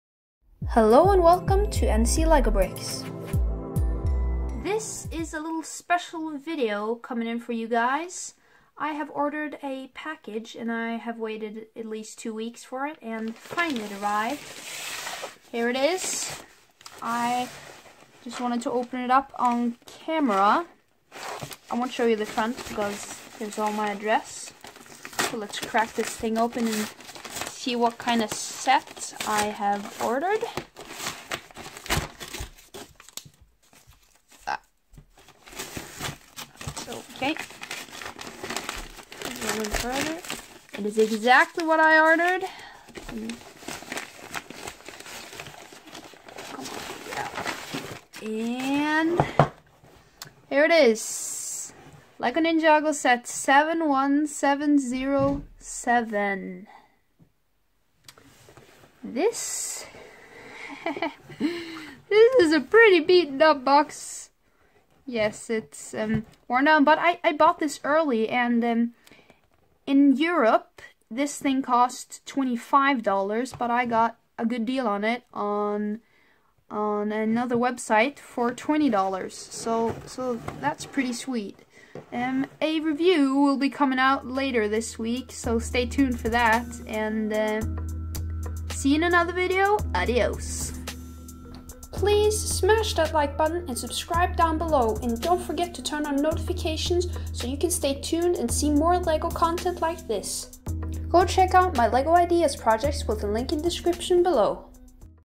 Mystery Lego Unboxing- Saved 5$ worth it?
I bought a sketchy listing online earlier this year, took two and a half weeks of waiting before the package arrived. Did I get what I ordered?

Note: All video's on this channel are made and produced by NC Lego Brix